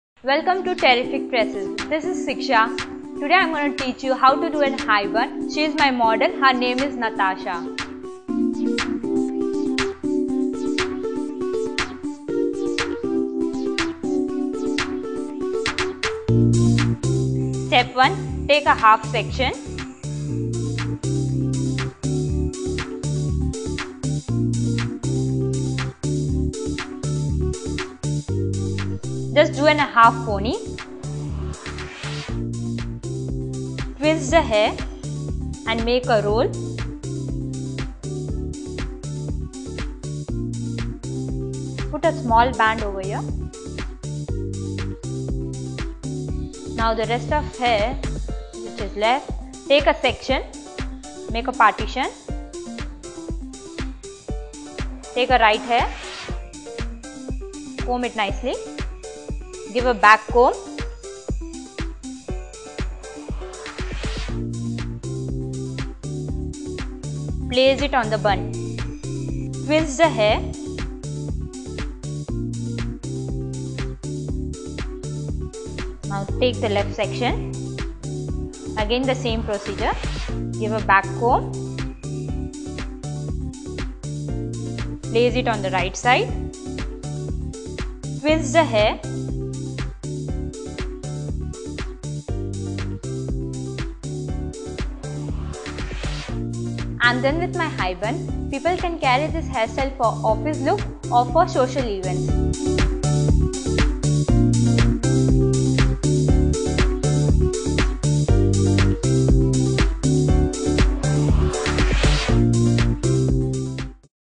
How to do a High Bun | Ventuno Terrific Tresses
A sexy, high bun is a hairstyle staple that every woman should know. This is an quick and easy High Bun tutorial. No sock or doughnut hair tool needed. It's great to wear to on a casual day, to work or on a date.